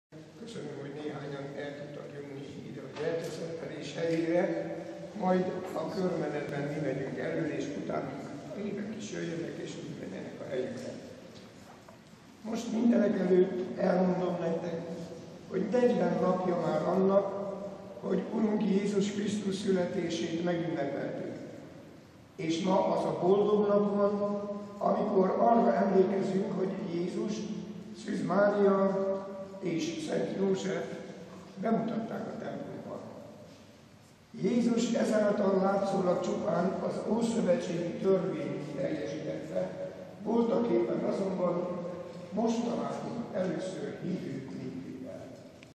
Gyertyaszentelő a római katolikus templomban
Ünnepre gyülekeztek a hívők hétfő este a Magyarok Nagyasszonya Társszékesegyház templomába, mert Jézus születésétől eltelt 40 nap, ami a megtisztulás ideje fiúgyermek születésekor. Ennek eltelte után térhet hívők közé az édesanya, és mutathatja be kisdedét. A mise elején megszentelték a gyertyákat, amik a fényességével távol tartja a rontó szellemeket.